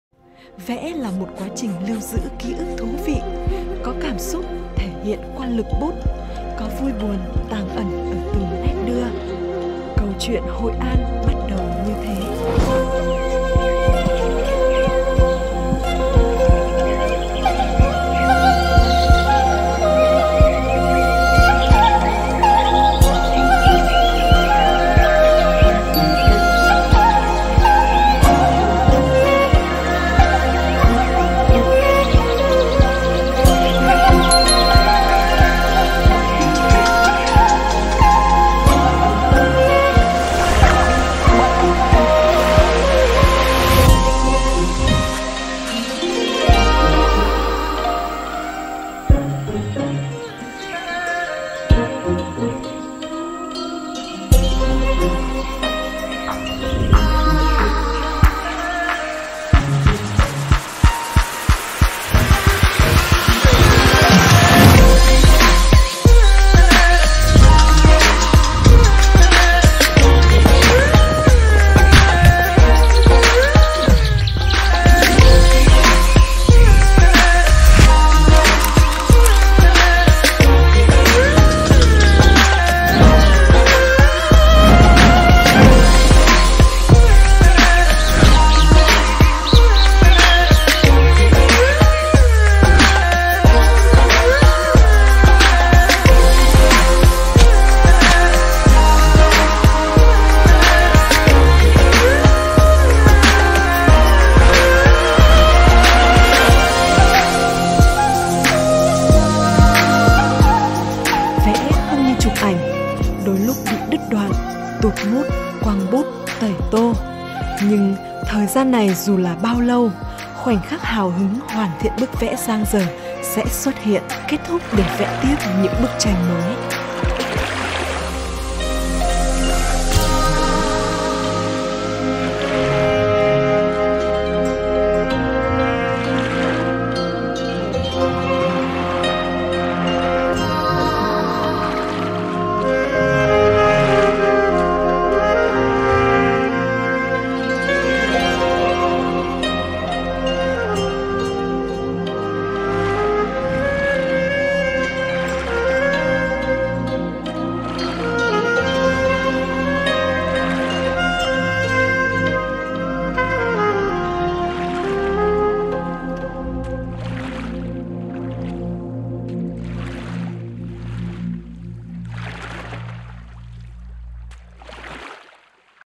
Hoi An Cinematic Short Film // Duc Dzus
Before the Covid-19 pandemic, we've had a rare chance to capture a deserted Hoi An. Hope you love it!

@Copyright Đức Dzú Quay Quay 2020